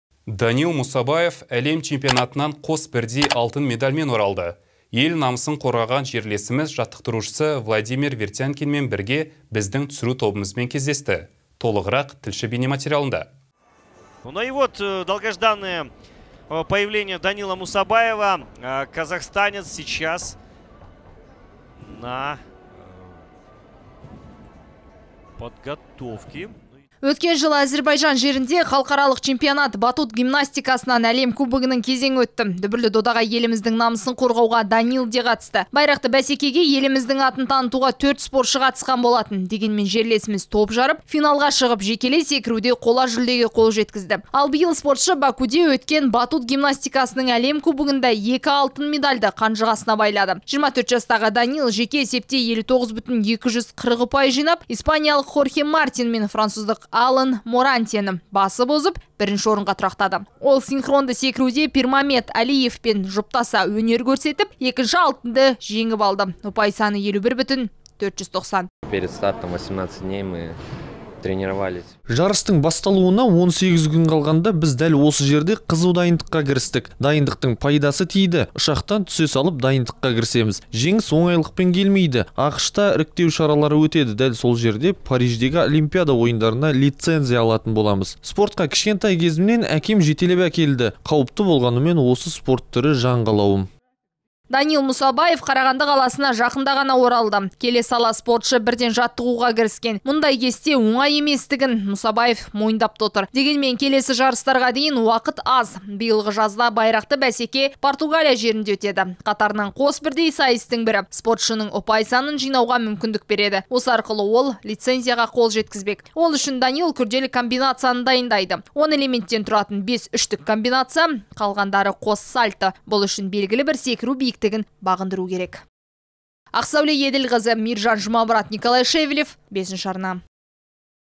Данил Мұсабаев әлем чемпионатынан қос бірдей алтын медальмен оралды.
Ел намысын қорғаған жерлесіміз жаттықтырушысы Владимир Вертянкинмен бірге біздің түсіру тобымызбен кездесті.